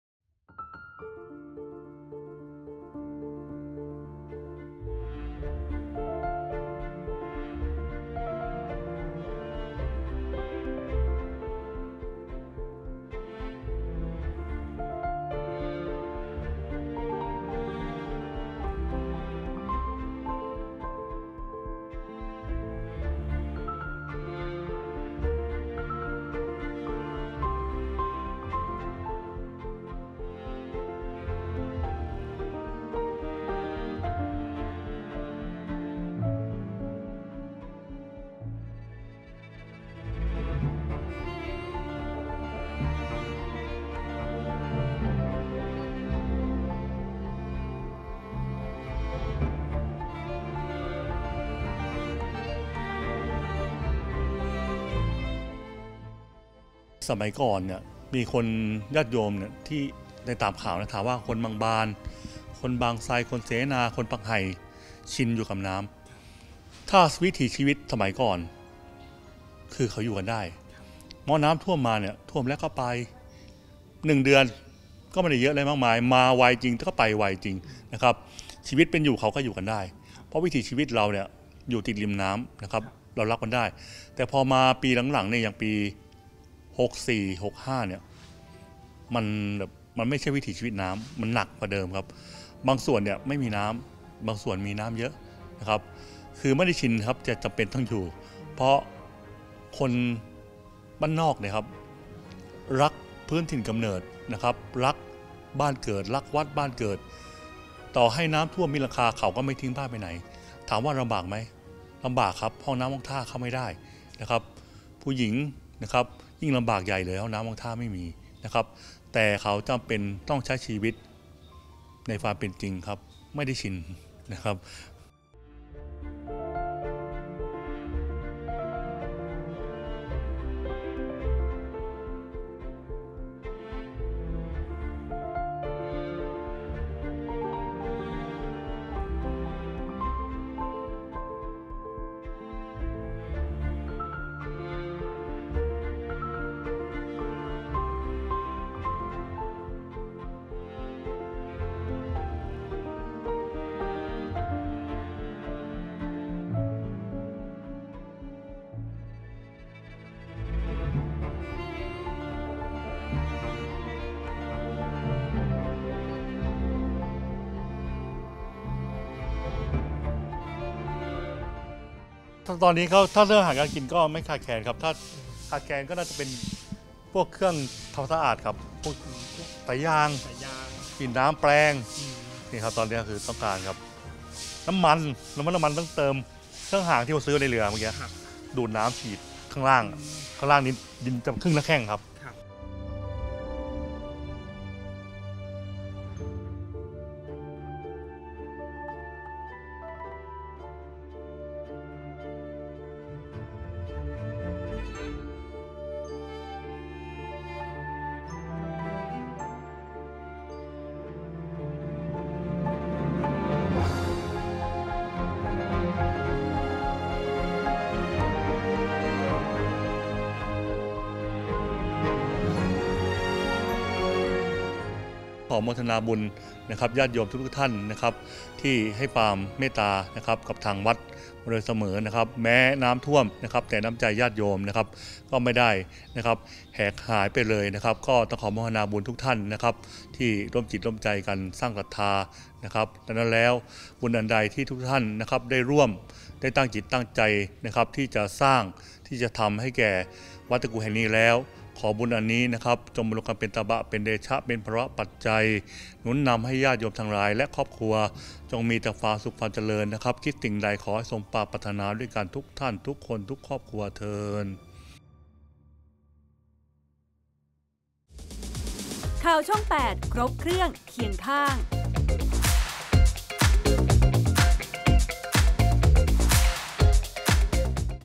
น้ำท่วมไม่ใช่สิ่งที่ชิน | ข่าวช่อง8
สมัยก่อน อ.บางบาล คือชุมชนริมน้ำ น้ำท่วมเป็นประจำ แต่ท่วมไม่ถึงเดือน แต่ปีนี้น้ำท่วมนานร่วม 2 เดือน สร้างความลำบากแก่ชาวบ้านมาก โดยเฉพาะ "วัดตะกู" ที่โบสถ์เสียหายหนัก